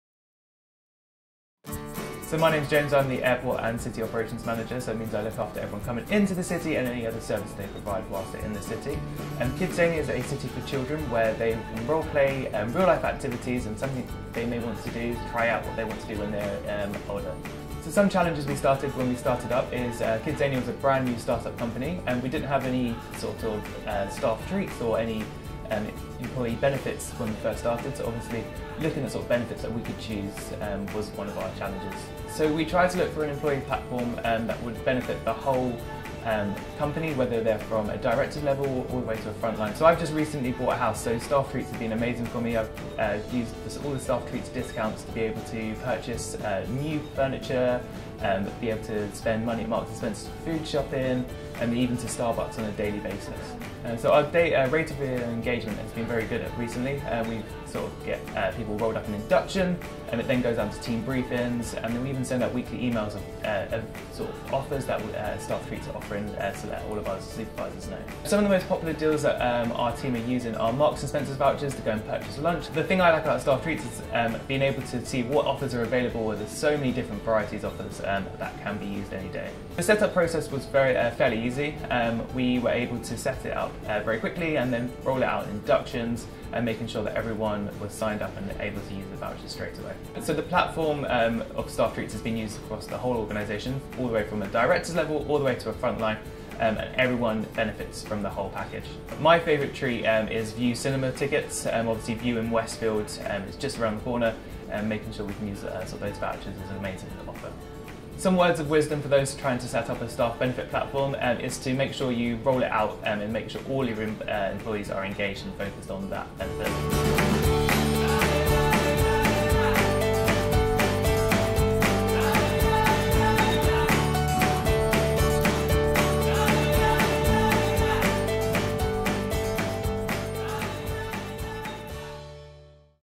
How KidZania London boosted employee engagement through Staff Treats
KidZania London partnered with Staff Treats to help improve their employee engagement rate, as well as attract and retain the right talent  especially with their active and diverse workforce.